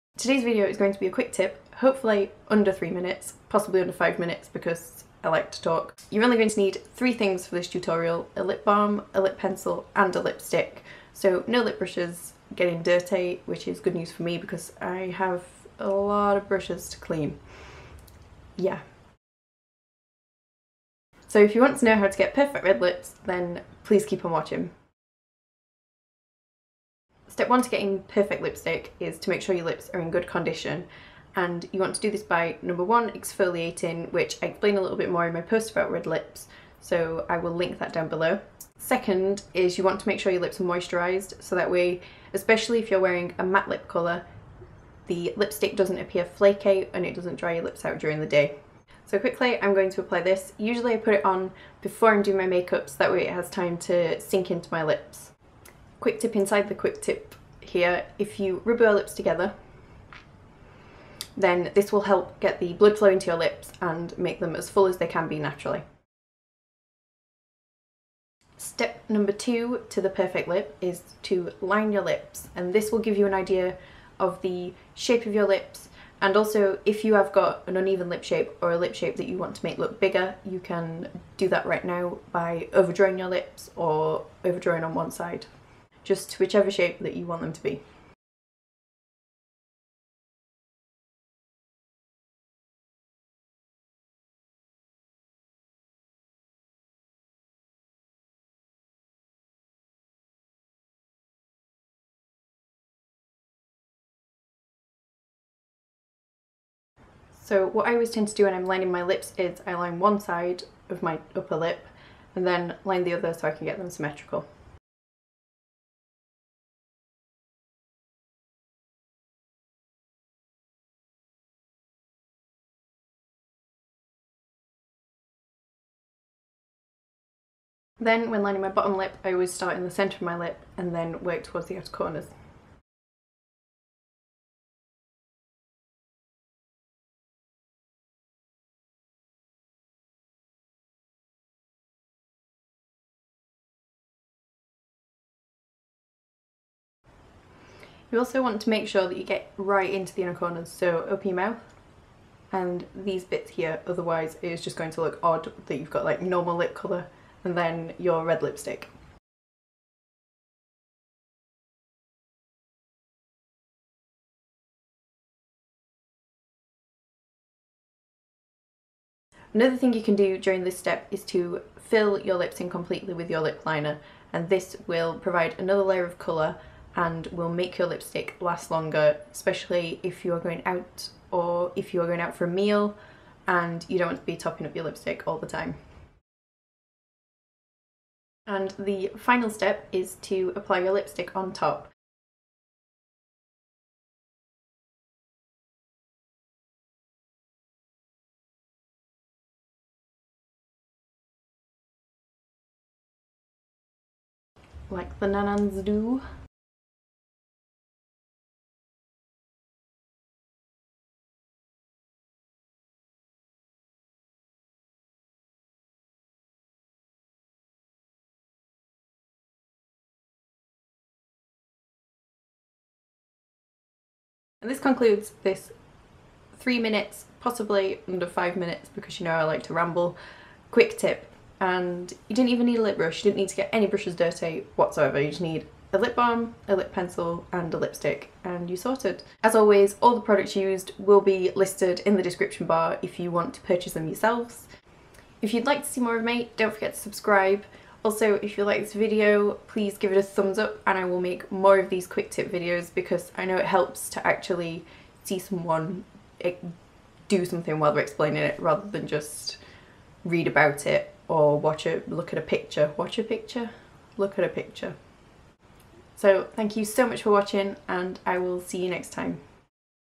Quick Tip: Perfect Red Lipstick | Phee's Makeup Tips
Hi guys! I thought I'd try something different, so here's a quick tip video to create a red lip look that's perfect for the festive season. I hope you enjoy it!

Products used:

Burt's Bees Beeswax Lip Balm
Illamasqua Creative Colouring Pencil
Melt Belladonna2 Lipstick